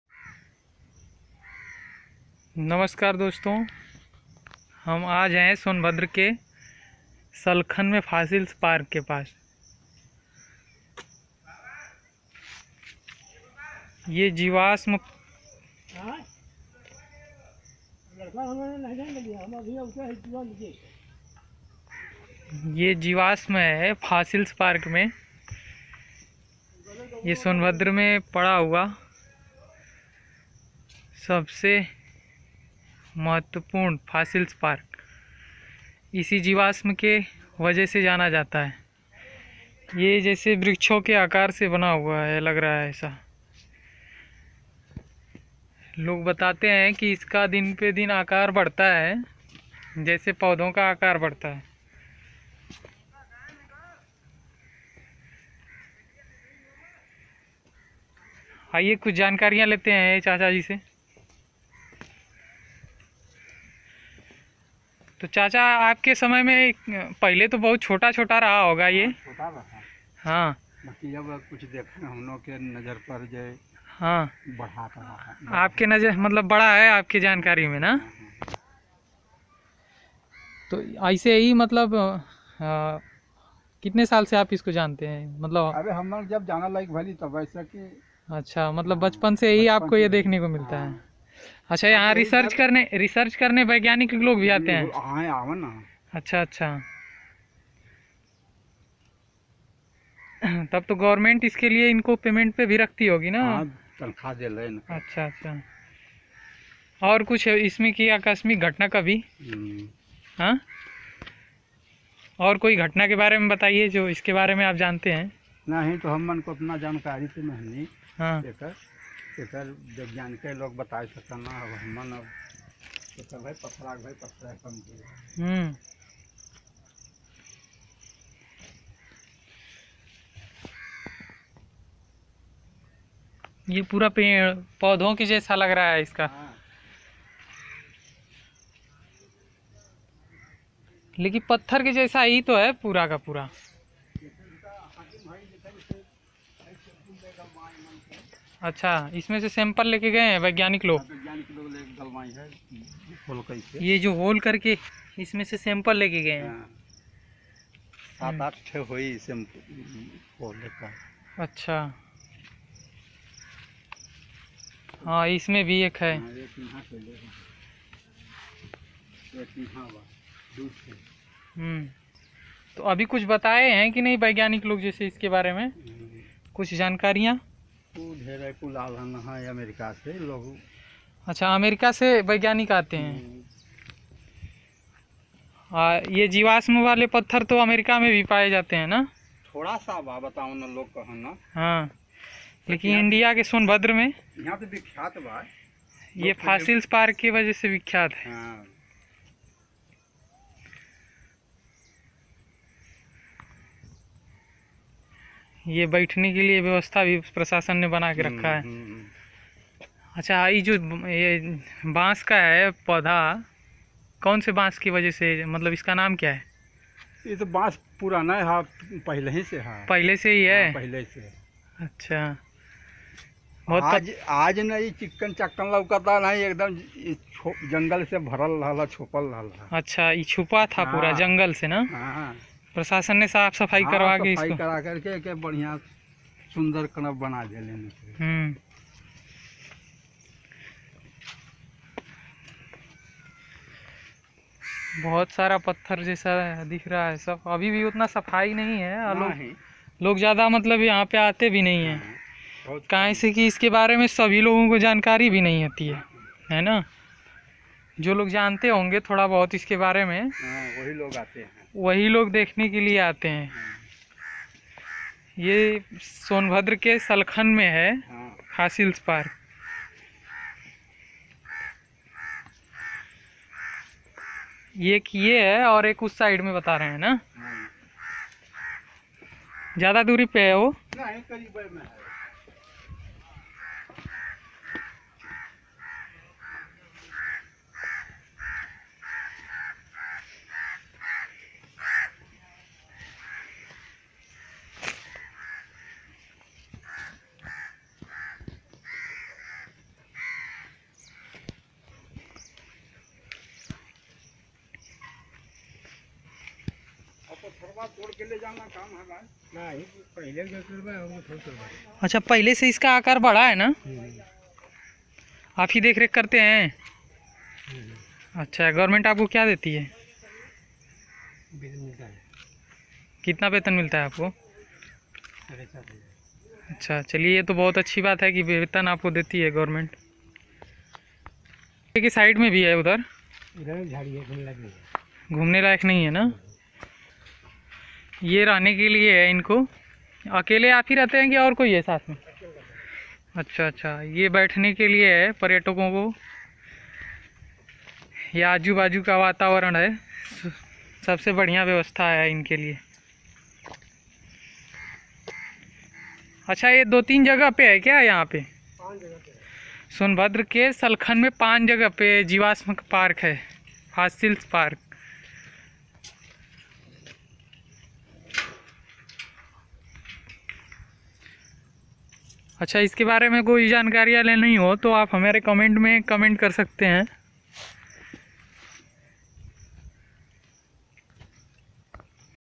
Fosils Park of INDIA. सलखन सोनभद्र में
वैज्ञानिकों द्वारा सोध से पता चला है कि ये जीवाश्म है जो जीवो और वृक्षों से बना है समुन्द्र के प्रभाव है